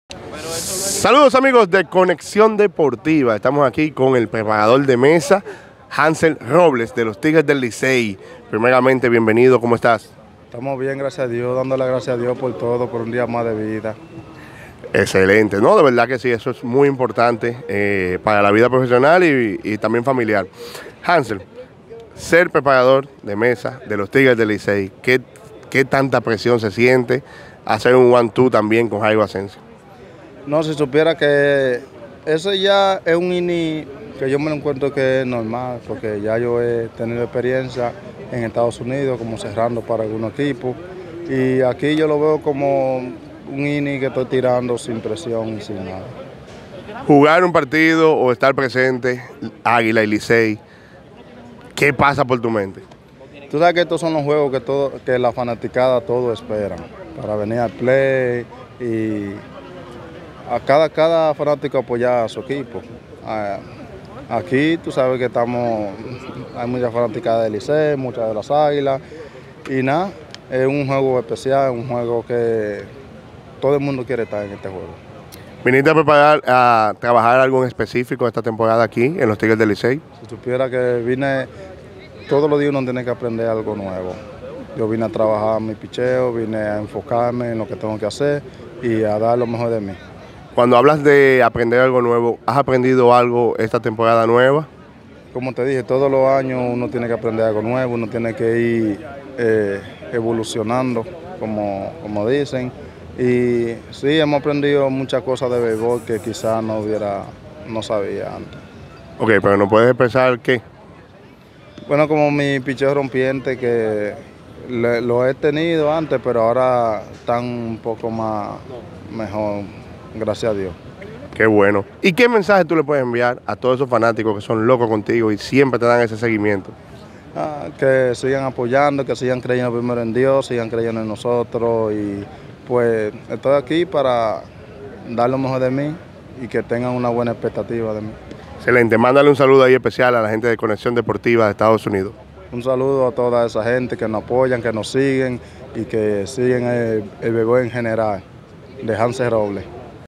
Hansel Robles ``Vine a trabajar mis pitcheo, a enfocarme y a dar lo mejor de mí´´
Este canal esta diseñado para el mundo del deporte con la finalidad de darles el mejor contenido del mundo desde la República Dominicana.